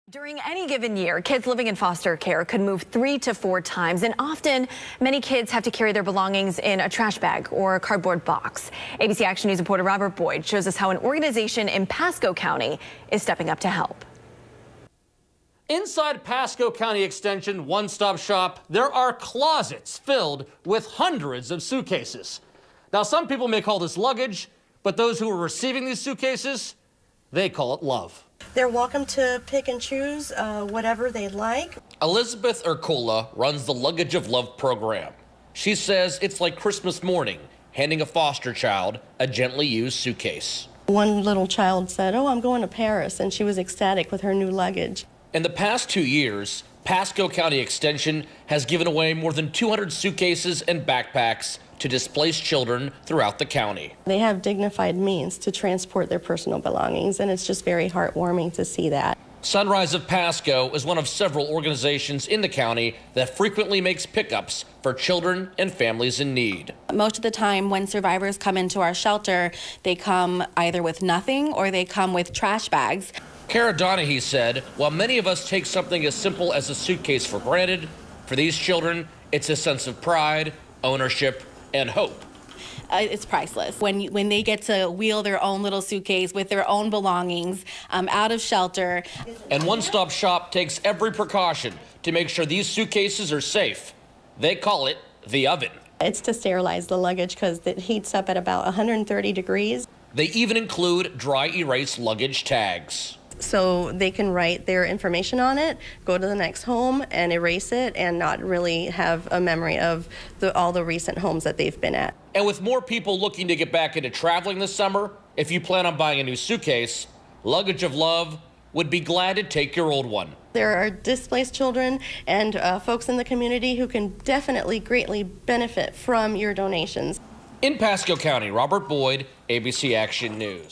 Pasco County Extension collecting luggage for foster children in need
In the last two years, the Pasco County Luggage of Love program has given out more than 200 donated suitcases to foster children in need.